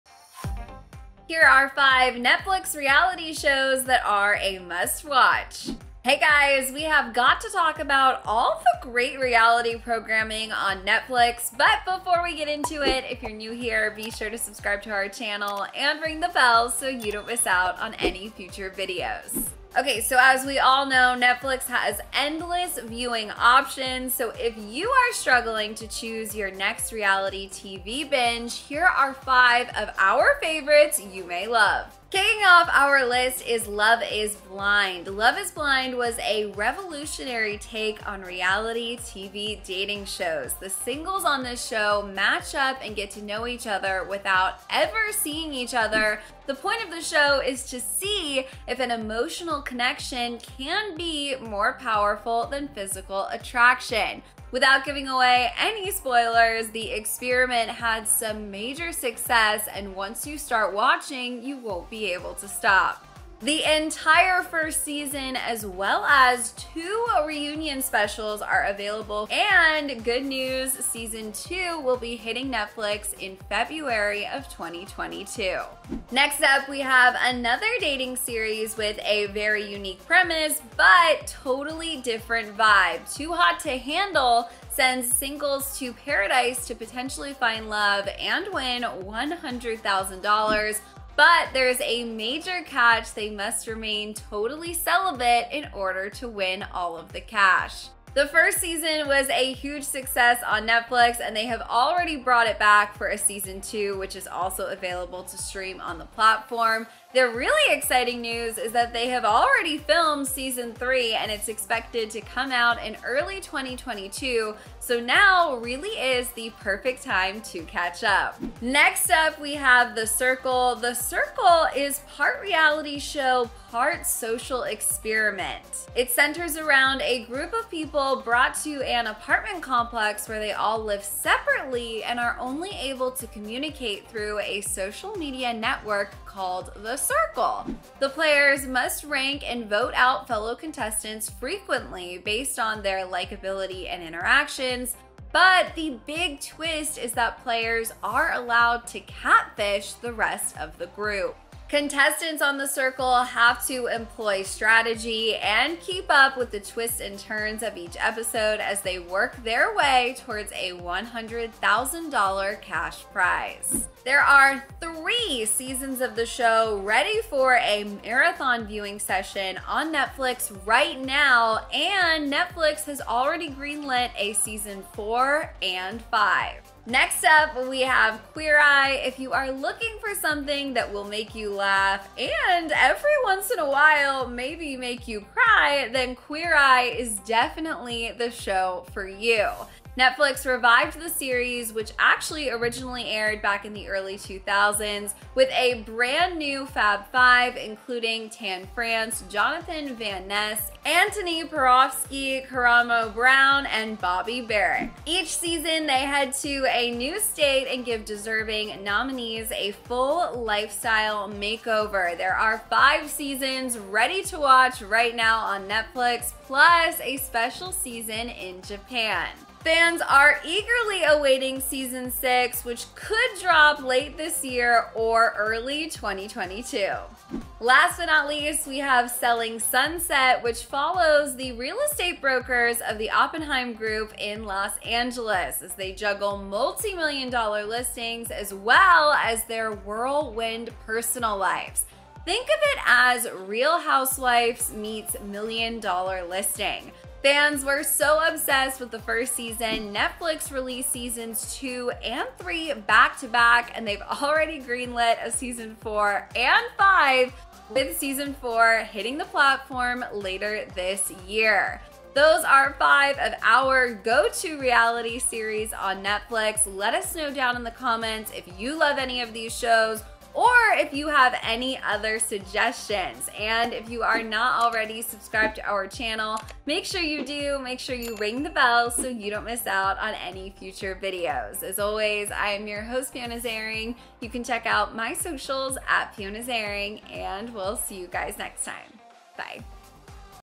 5 Best Reality Shows On Netflix
CLICK HERE For More Reality TV News & Reactions!

Here are 5 Netflix reality shows that are a must watch!

Hey guys! We’ve got to talk about all the great reality content on Netflix, BUT…

Ok so we all know Netflix has endless viewing options so if you’re struggling to pick your next reality tv binge here are 5 of our favorites you may love!

Kicking off our list is Love Is Blind! Love is Blind was a revolutionary take on reality dating shows! The singles on the show match up and get to know each other without EVER seeing each other-- the point of the show is to see if an emotional connection can be more powerful that physical attraction!

Without giving away any spoilers the experiment had some major success and once you start watching you won’t be able to stop!

The entire first season as well as 2 reunion specials are available for viewing on the platform! And season 2 will hit the platform in February of 2022.

Next up we have another dating series with a unique premise but totally different vibe! Too Hot To Handle sends singles to paradise to potentially find love and win $100,000 but there’s a major catch they must remain TOTALLY celibate in order to win the cash!!

The first season was a smash success and Netflix brought it back for a second season which is already available to watch on the platform!

The exciting news is Netflix has already filmed season 3 and it's expected to come out in early 2022 so now is the perfect time to catch up!

Next up we have The Circle! The Circle is part reality show, part social experiment and centers around a group of people brought to an apartment complex where they all live separately, and are only able to communicate through a social media network called The Circle!

The players must rank and vote out fellow contestants frequently based on their likability and interactions, but the big twist is players are allowed to catfish the rest of the group.

Contestants on the circle have to employ strategy and keep up with the twists and turns of each episode as they work their way towards a $100,000 cash prize!

There are 3 seasons of the show ready for a viewing marathon right now on Netflix and Netflix has already greenlit a season 4 AND 5!!!

Next up we have Queer Eye! If you’re looking for something feel good guaranteed to make you laugh AND every once and a while maybe even cry Queer Eye is for you!

Netflix revived the series which originally aired back in the early 2000s with a brand new Fab 5 including Tan France, Jonathan Van Ness, Antoni Porowski, Karamo Brown and Bobby Berk!

Each season they head to a new state and give deserving nominees a full lifestyle makeover! There are 5 seasons ready to watch on Netflix plus a special season in Japan!

Fans are eagerly awaiting season 6 which could drop late this year or early in 2022!

Last but not least we have Selling Sunset! Which follows the real estate brokers of the Oppenheim Group in Los Angeles, as they juggle multi-million dollar listings as well as their whirlwind personal lives! Think of it as real housewives meets million dollar listing!

Fans were so obsessed with the first season Netflix released season 2 & 3 back to back and they’ve already greenlit season 4 & 5 with 4 hitting the platform later this year!

Those are 5 of our go to reality series from netflix! Let us know if you’ve watched any of these and leave us your recommendations down in the comments!